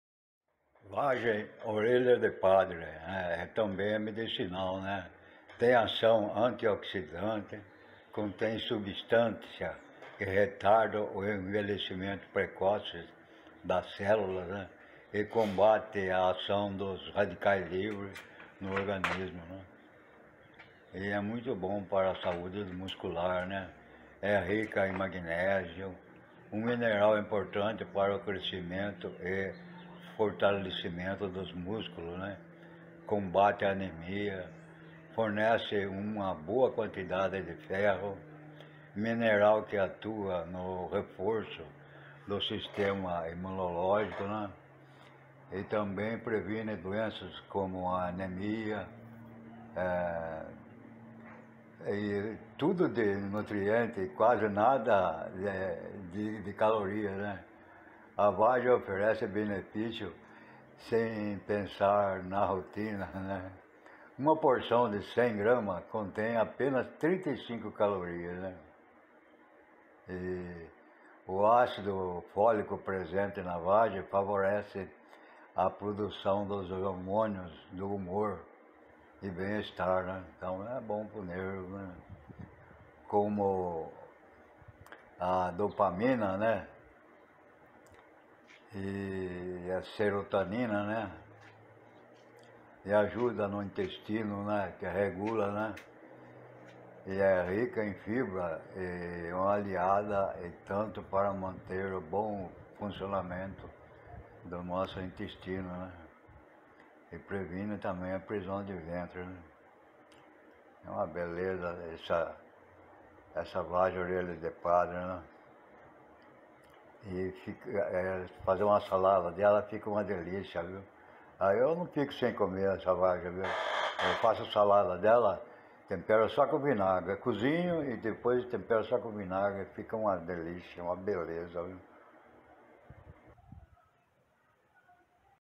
Orelha de padre e seus benefícios
Nome cientifico.: Científicos. Lablab purpureus(L)
Nomes populares.: Orelha de padre ou lab lab, mangalô, feijão de pedra
vagem orelha de padre benefícios
Tem ação antioxidante: contém substâncias que retardam o envelhecimento precoce das células e combatem a ação dos radicais livres no organismo e para saude muscular: é rica em magnésio, um mineral importante para o crescimento e fortalecimento dos músculos. Combate à anemia: fornece uma boa quantidade de ferro – mineral que atua no reforço do sistema imunológico, e também previne doenças como a anemia. Tudo de nutrientes, quase nada de calorias: a vagem oferece benefícios sem pesar na rotina: uma porção de 100 gramas contém apenas 35 calorias. Xô mau humor: o ácido fólico presente na vagem favorece a produção dos hormônios do humor e bem-estar, como a dopamina, noradrenalina, homocisteína e serotonina. Intestino regulado: rica em fibras, é uma aliada e tanto pra manter o bom funcionamento do intestino e prevenir a prisão de ventre.
Previne o diabetes: A vagem pode ajudar a controlar os níveis de açúcar no sangue, reduzindo as possibilidades do indivíduo desenvolver o diabetes.

Rey das Plantas